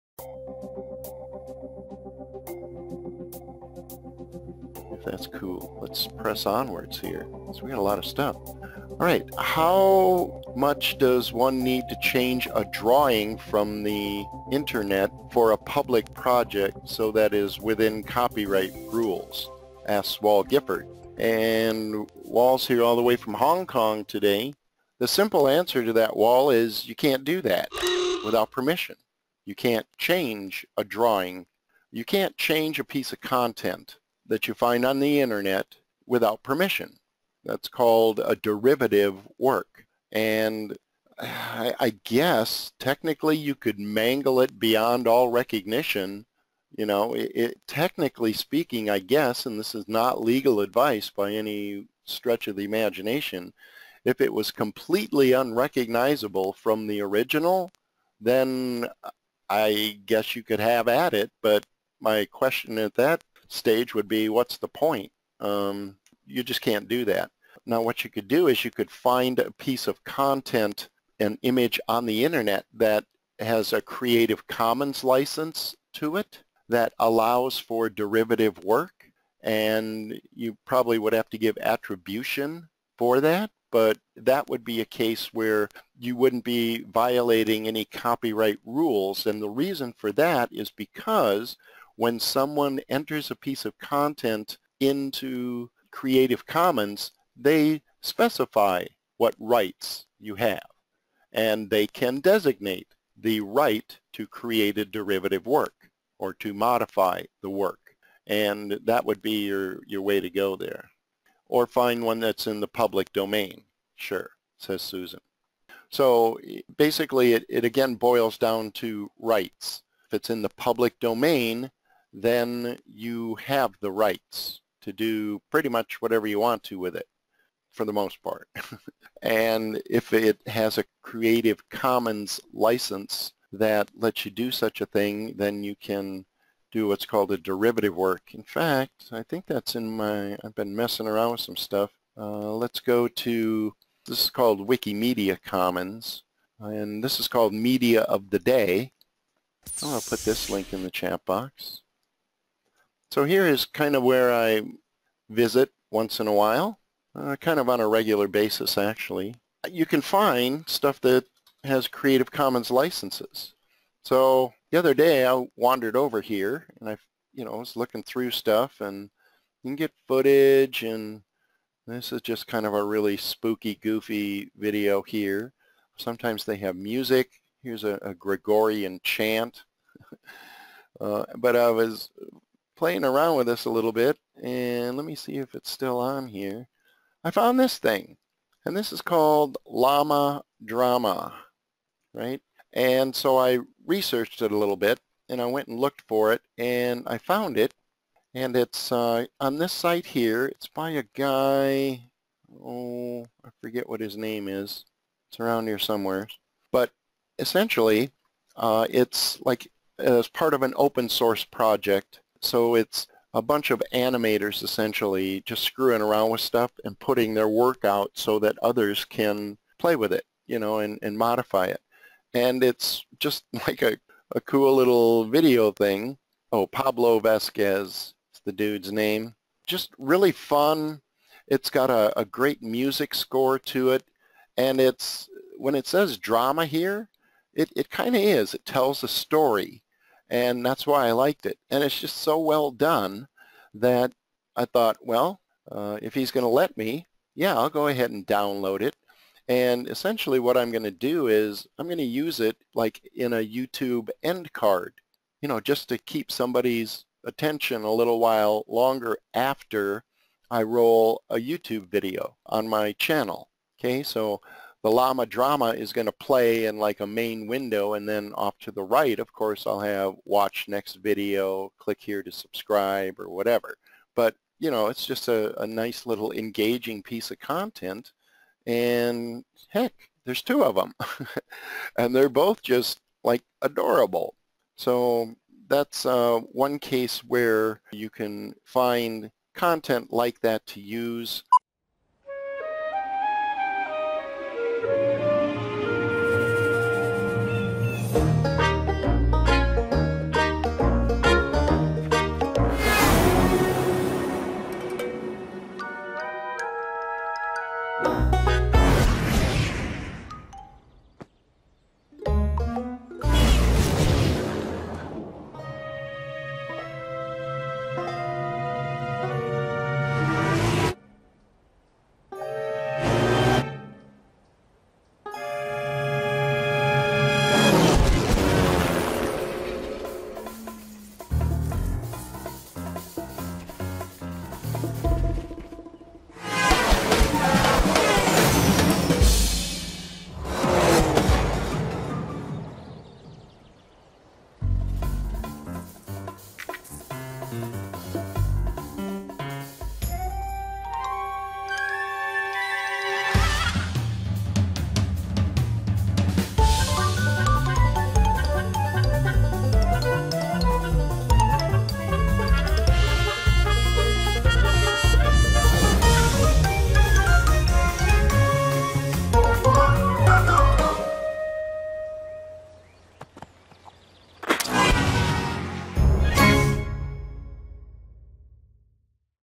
Creative Commons Wikimedia Of The Day
Here's a question asked during the webinar about if you can modify images & stuff you find on the internet to use in your videos. Short answer: Nope. But you can find things that do give the rights to modify.

Thanks!
Lon Naylor